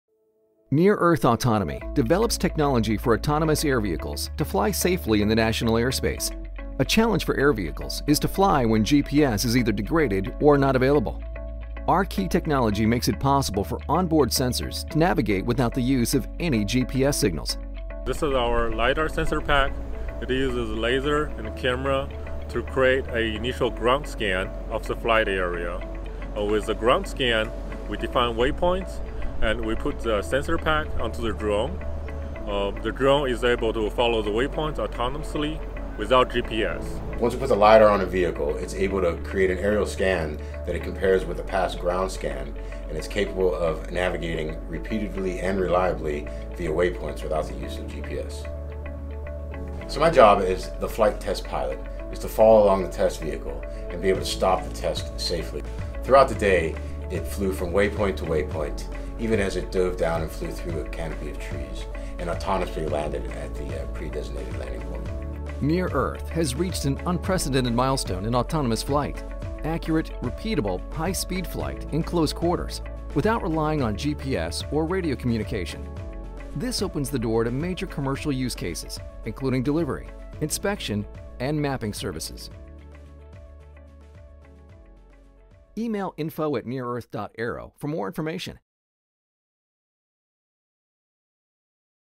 Precision Autonomous Flight Without GPS
Near Earth Autonomy develops technology for autonomous air vehicles to fly safely in the national airspace. In this video, our LIDAR navigation system allows a drone to fly through waypoints registered to 3D landmarks at high speeds without the use of GPS or ground markers.